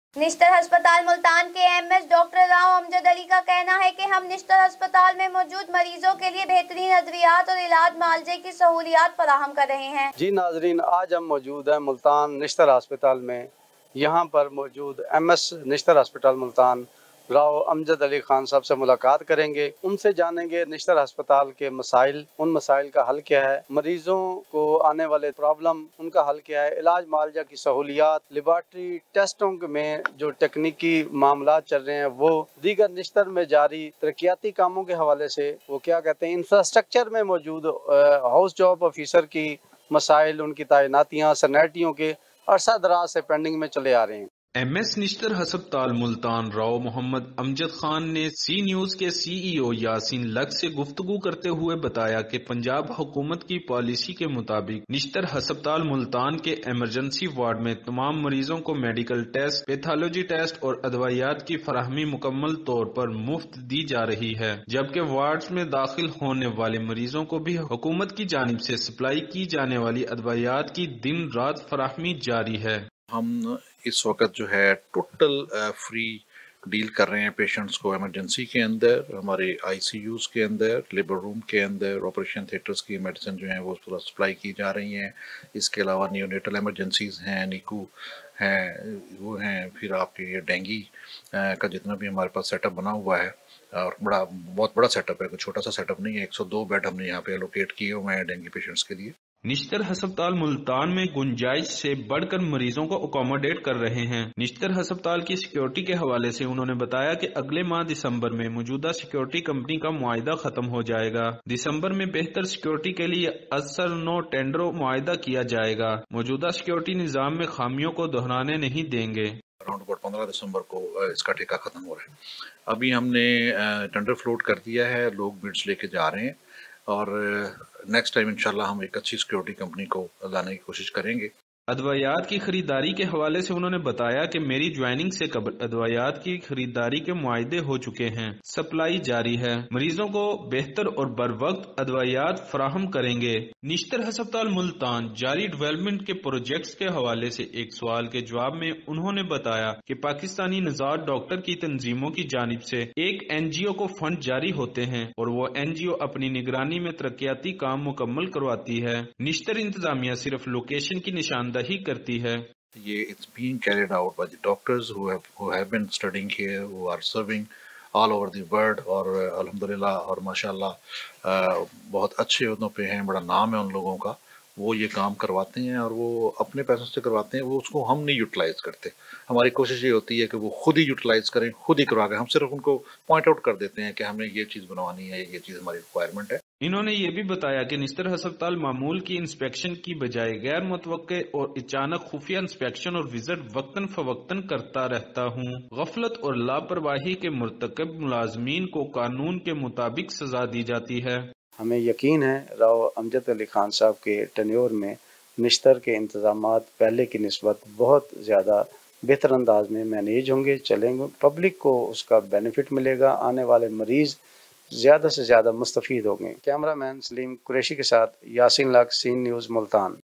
Nishtar Hospital Multan Situation | special report | exclusive interview Ms Nishtar | 26 November
Nishtar Hospital is a teaching hospital of Nishtar Medical University, located in Multan, Pakistan. It is a tertiary care hospital, serving a large population of South Punjab.
In October 2022, it was discovered that multiple abandoned dead bodies which remained unclaimed, were stored on the roof of the hospital instead of proper burial. Later, HOD Anatomy Department revealed that the bodies couldn't be buried because of policy of handover of corpses to Police, which didn't take place owing to delay.[6] horrific videos and images of multiple deceased people found on the roptop of the Hospital, Multan in a bad condition circulated on social media. After the pictures and videos came out, questions arose on social media that whose bodies are these and why they are lying helpless.